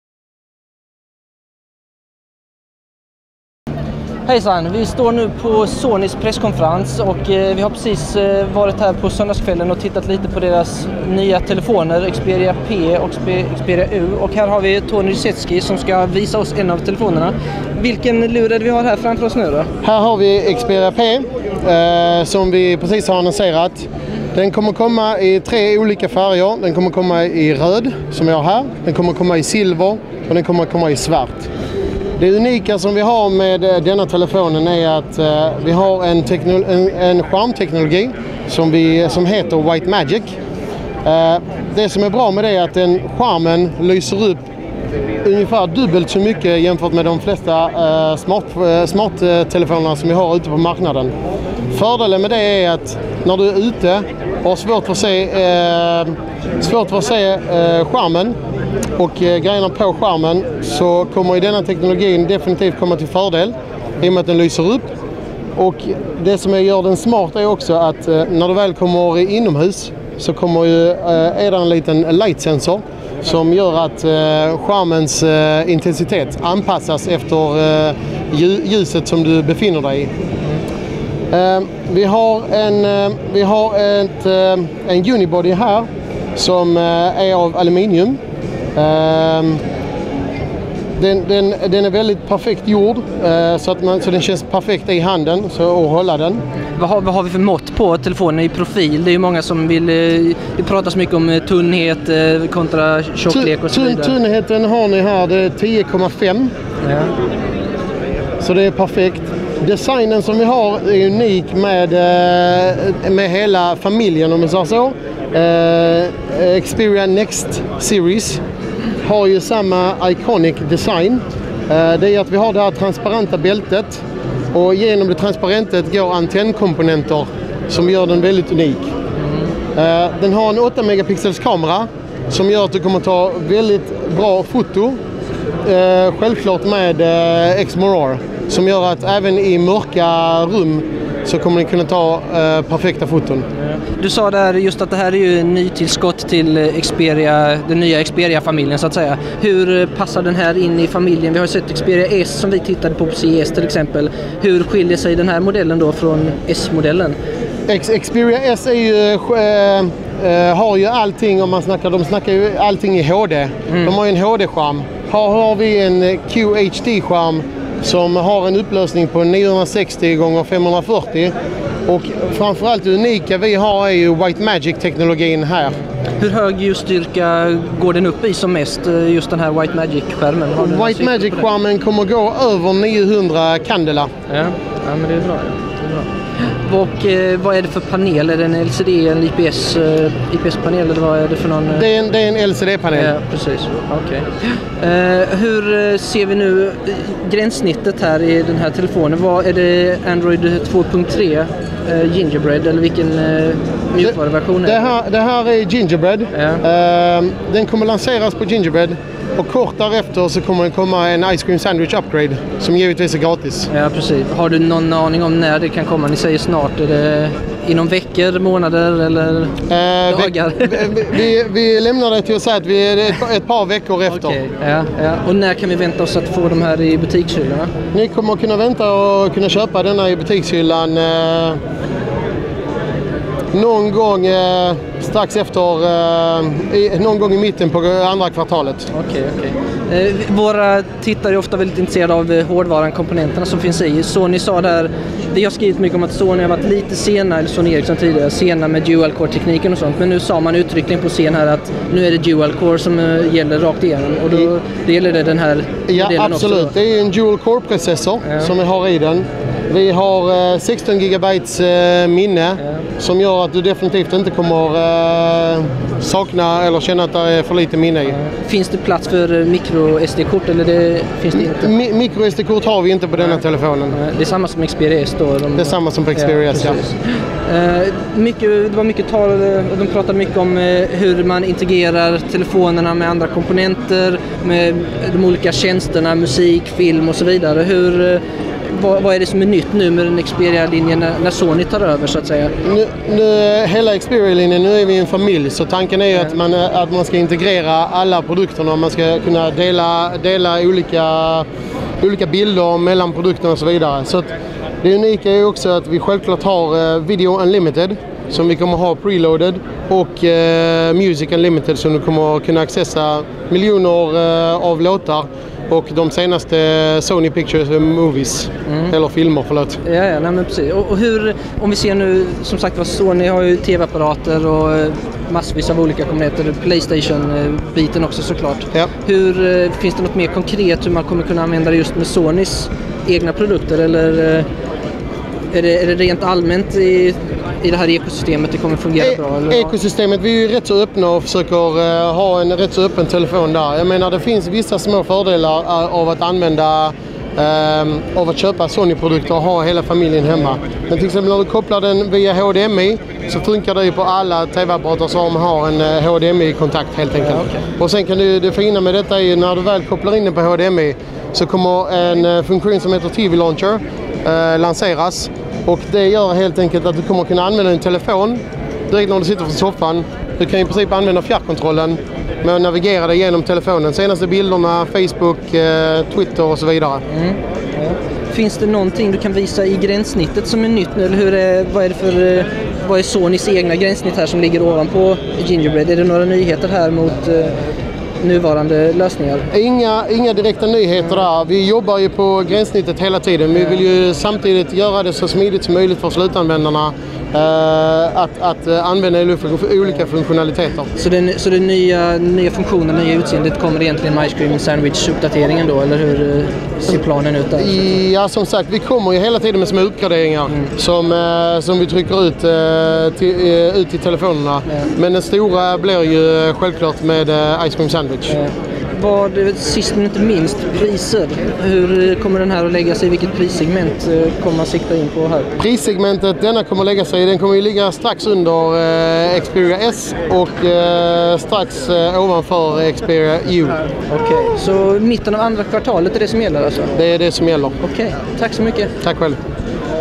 Sony Xperia P med White Magic-teknik för hög skärmljusstyrka
Sonys nya Xperia NXT-serie ser ut att fokusera på en enhetlig design, fullproppad med diverse olika tekniker man knappt kommer ihåg namnet på! Xperia P ser överlag ut som en bra smartphone, som kommer anlända i färgerna svart, röd och silver. Vi hoppas också att deras White Magic-teknik lever upp till vad som utlovas!